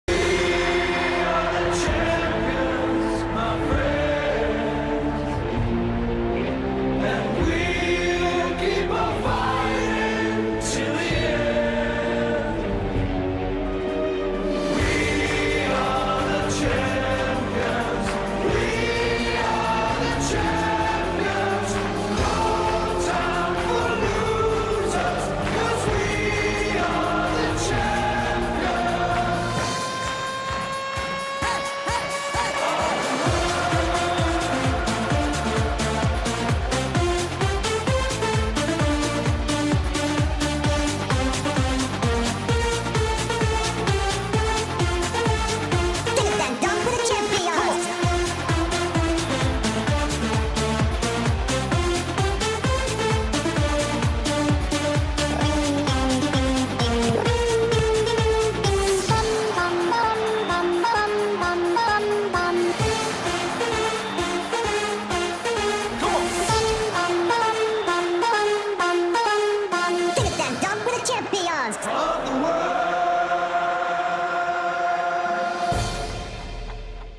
The Champions of GaladariBrothers for Dubai30x30
Let's give a big round of applause for the GaladariBrothers Champions for DubaiFitnessChallenges2019 at the GaladariFestival Day, Friday, 15th November 2019.